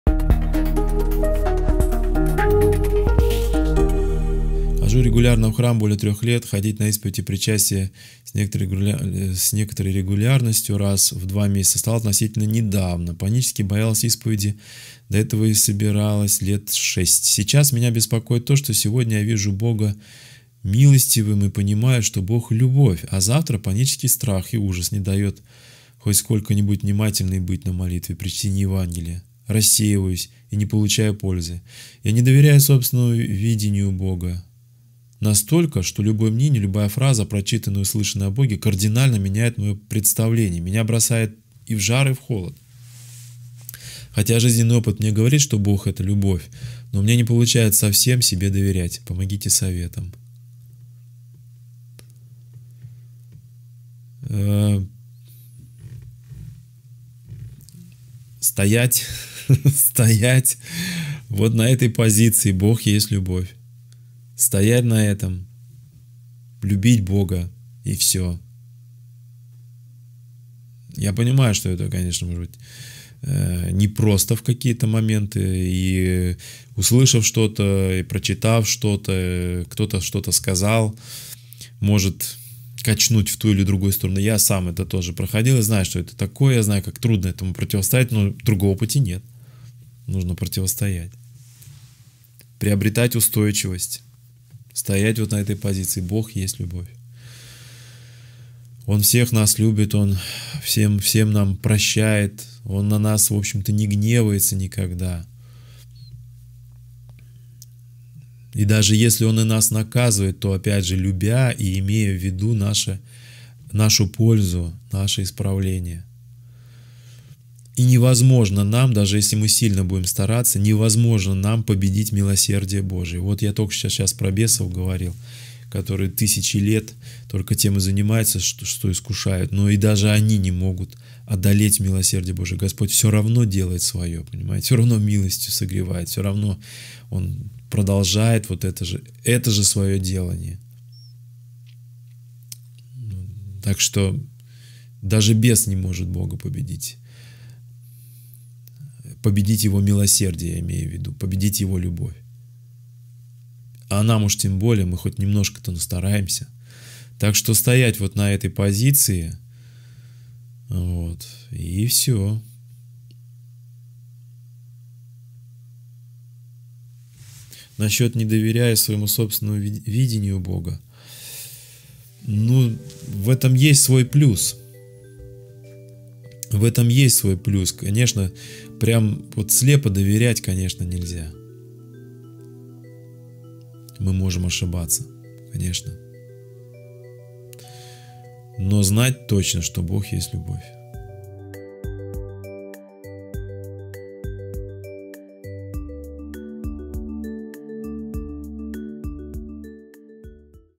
Бог есть любовь. Вера и сомнение
Понимаю что Бог есть любовь, что Бог милостив, верю в это, но и одолевают сомнения и страх. Не доверяю себе и своему восприятию Бога. О вере, неверии и сомнении.